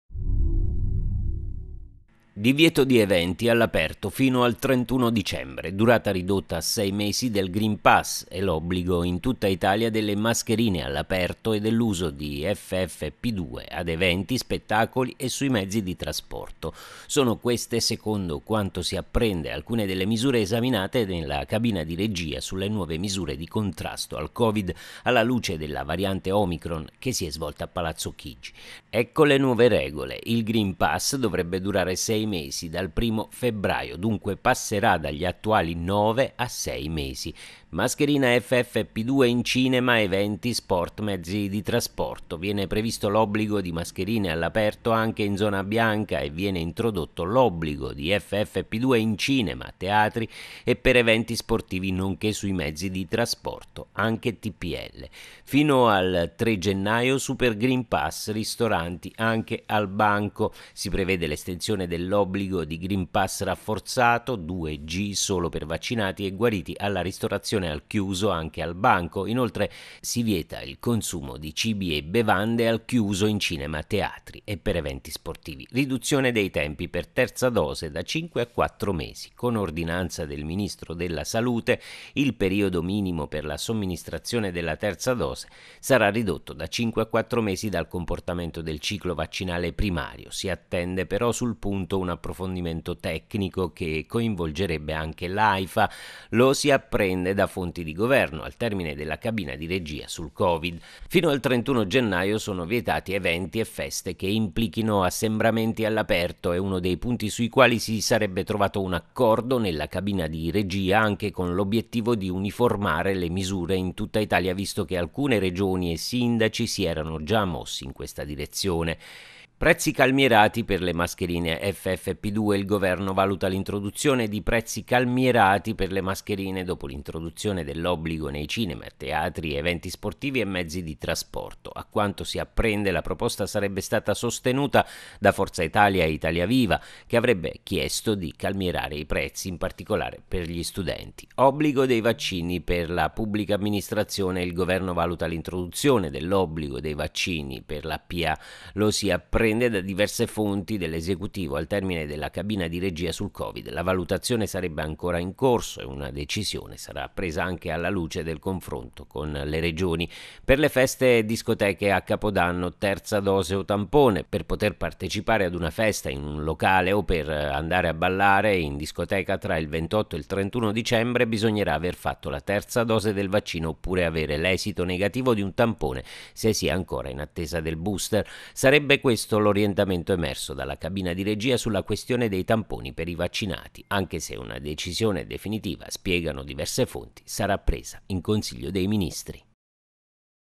Covid-19, Italia verso nuove regole : Ecco le nuove misure di contrasto al Covid
Divieto di eventi all’aperto fino al 31 dicembre, durata ridotta a 6 mesi del Green Pass e l’obbligo in tutta Italia delle mascherine all’aperto e dell’uso di Ffp2 ad eventi, spettacoli e sui mezzi di trasporto.